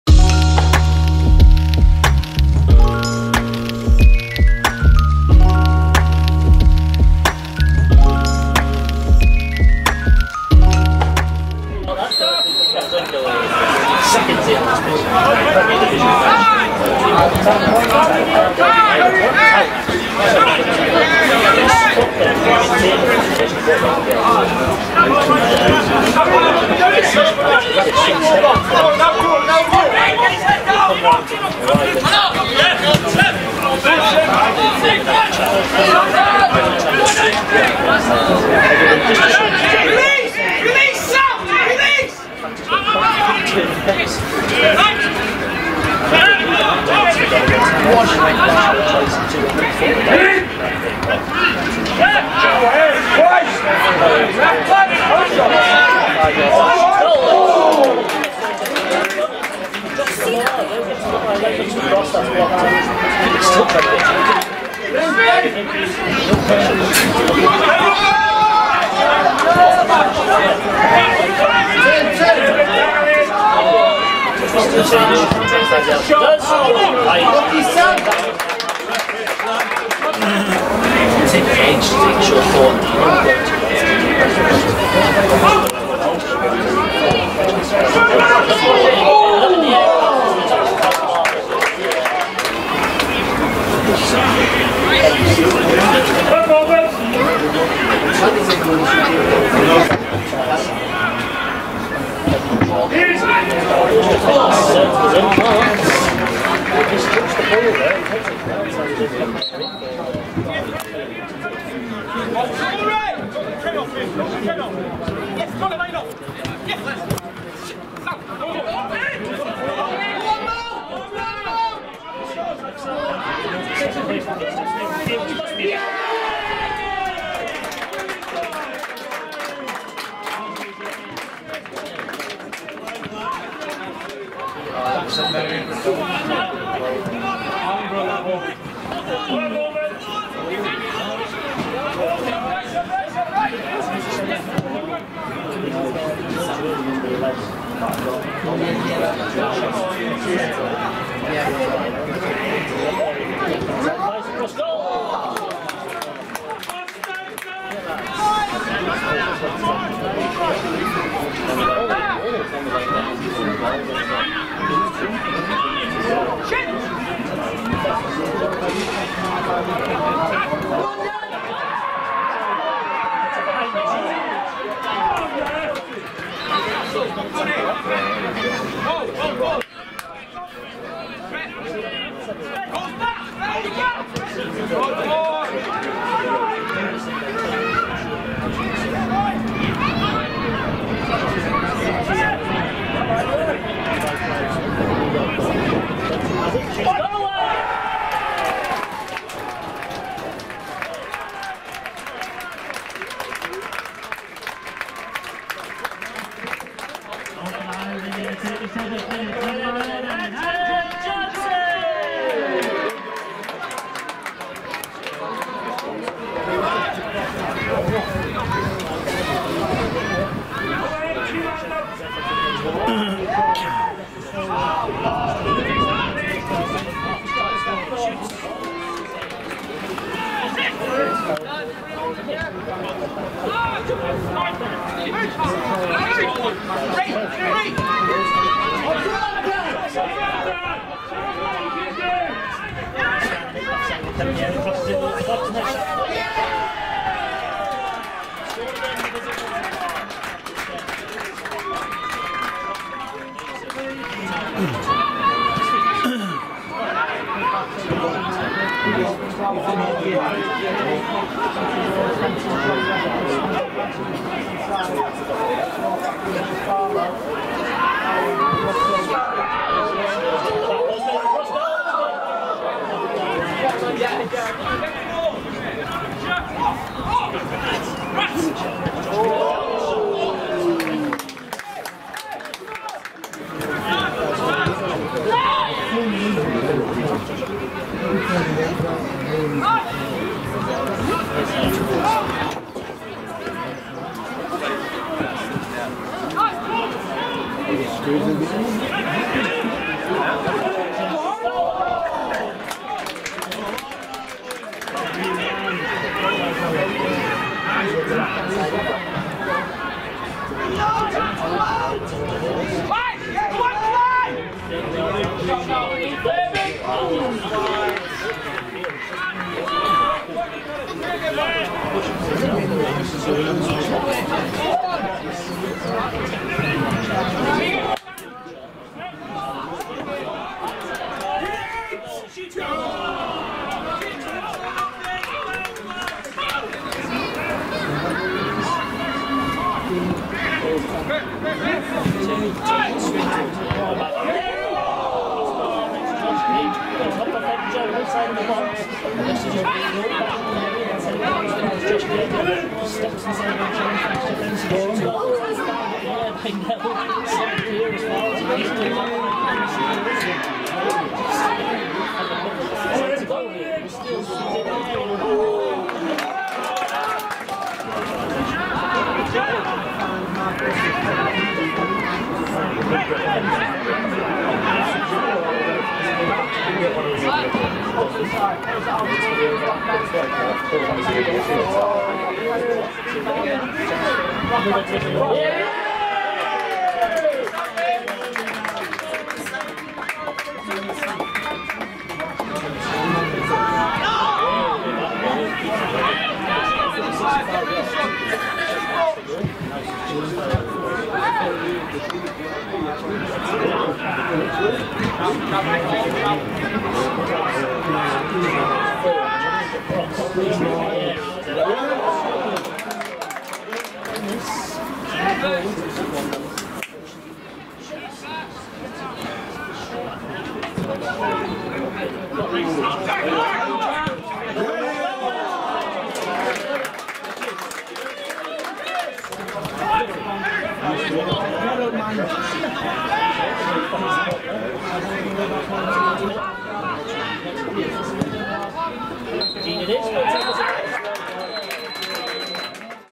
Highlights: Morpeth Town 1 Warrington Rylands 3 (23/24)
📽️ Highlights from a 3-1 loss at home to Warrington Rylands in the Pitching In NPL Premier Division:

⚽️ Andrew Johnson (37')

⚽️ Ollie Shenton (22')
⚽️ Ben Hardcastle (45')
⚽️ Sam Burns (79')